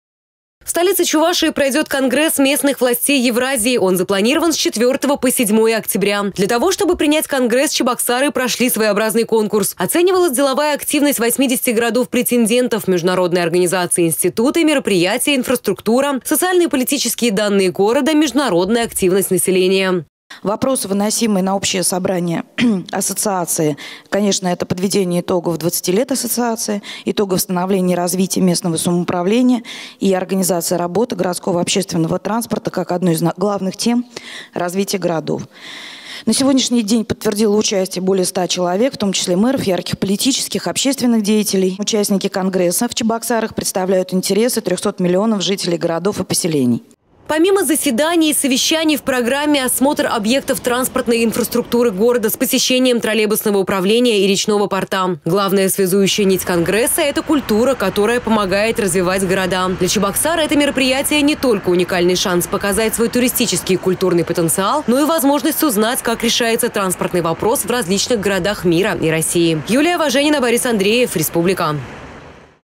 В столице Чувашии пройдет конгресс местных властей Евразии.
Он запланирован с 4 по 7 октября. Для того, чтобы принять конгресс,  Чебоксары прошли своеобразный конкурс.